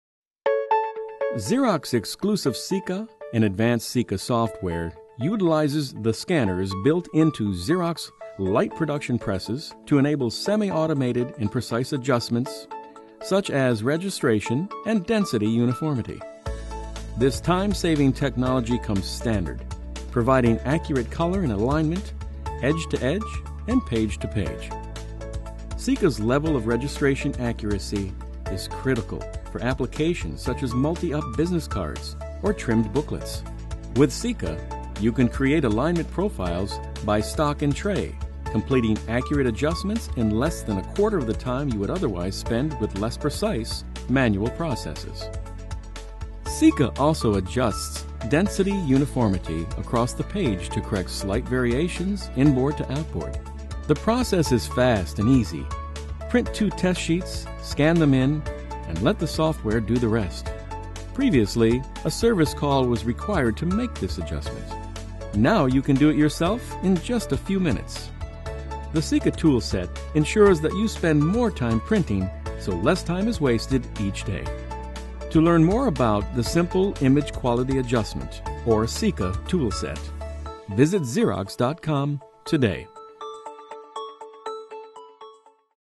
Xerox SIQA Toolset
Video promoting the new SIQA Software from Xerox.